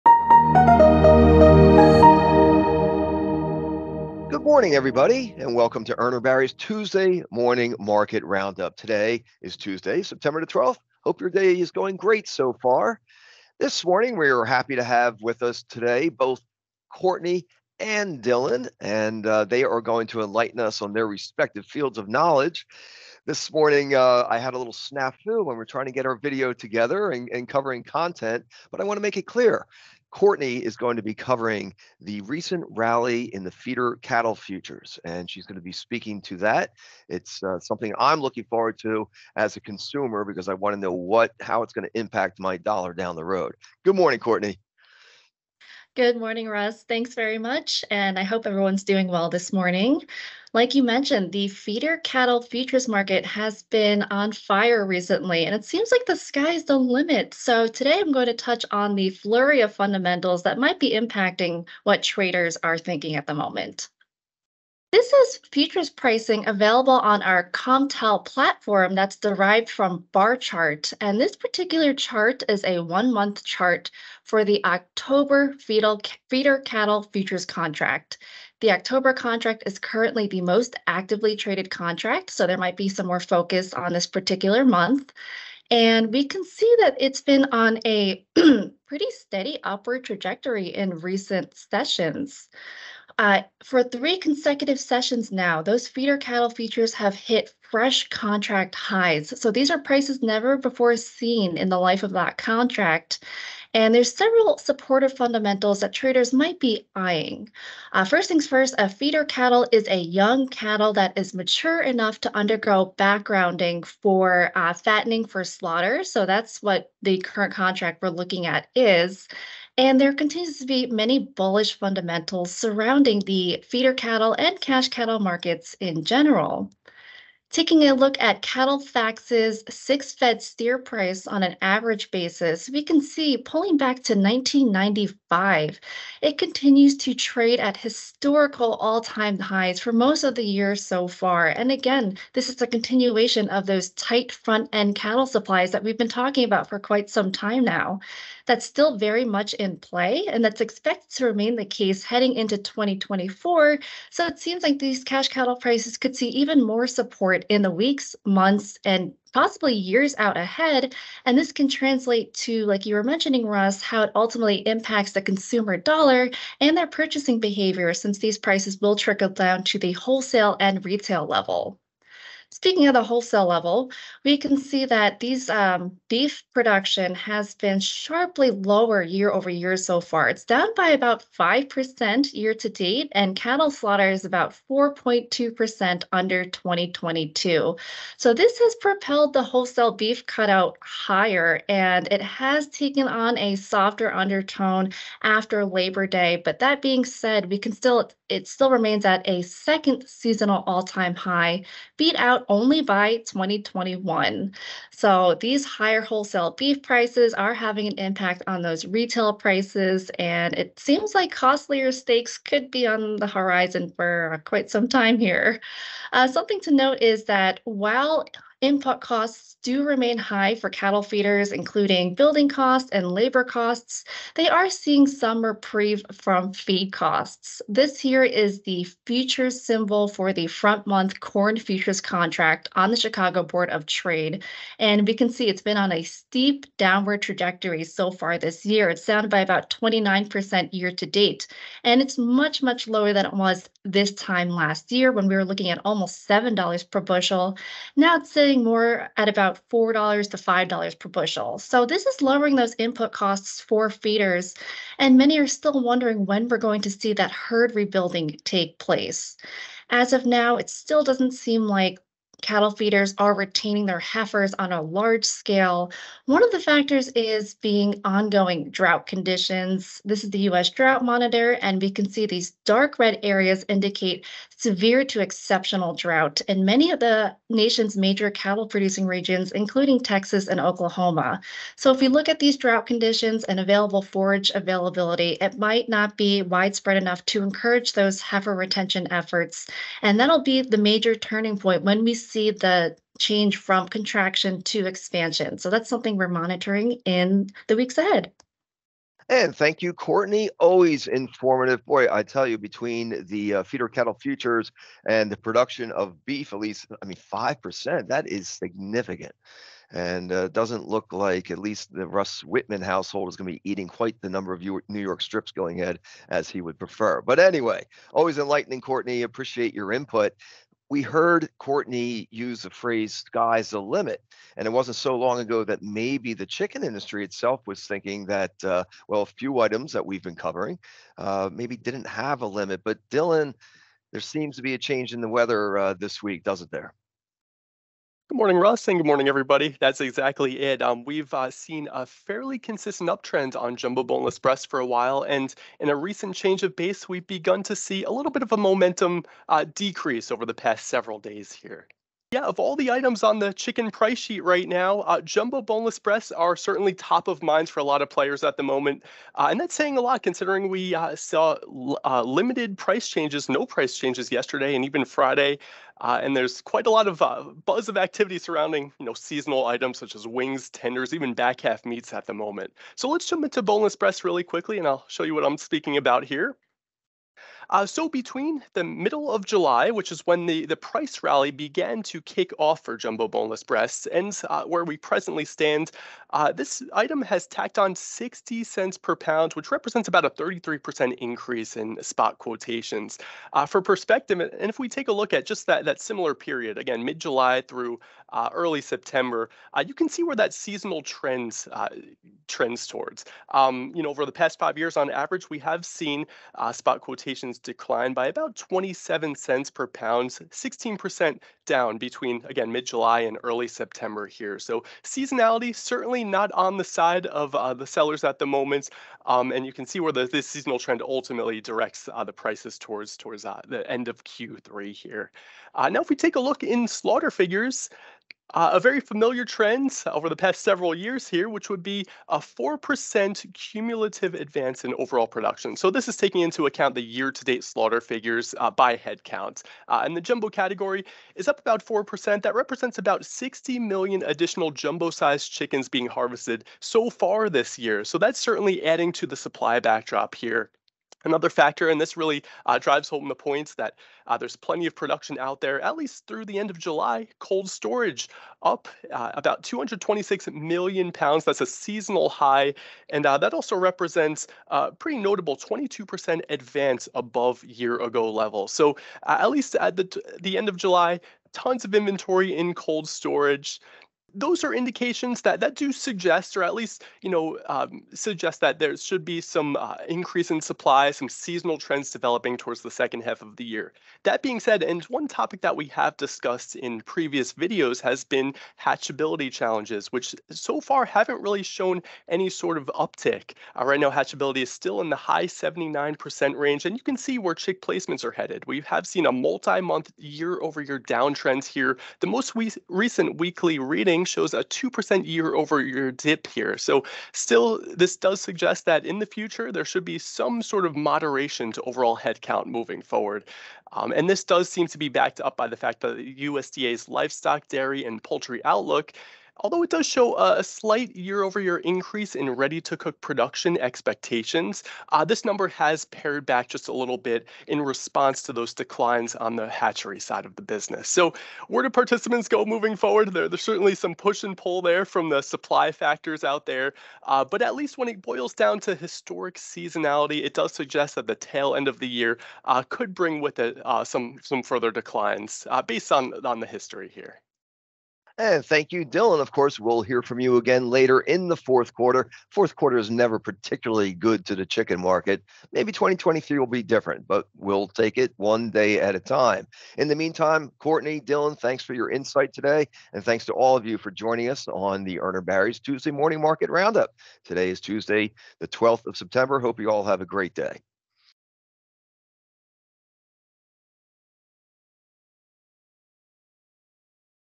Feeder cattle futures and boneless skinless chicken breasts
Watch Urner Barry's reporters discuss feeder cattle futures highs and seasonal trends for boneless skinless chicken breasts.